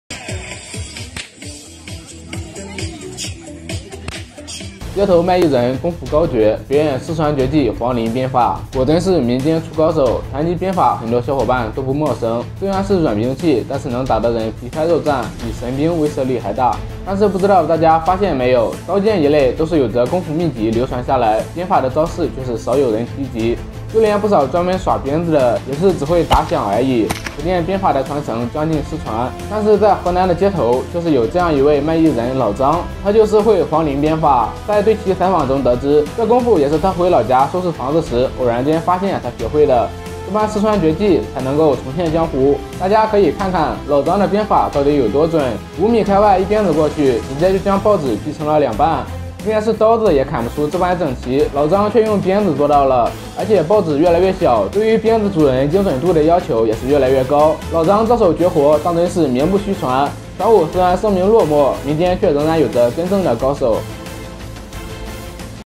艺高人胆大！街头艺人挑战极限，我感觉拿报纸的小伙两腿在发抖！
黄鳞鞭法再现江湖，五米开外一鞭子劈断报纸，果真是民间出高手！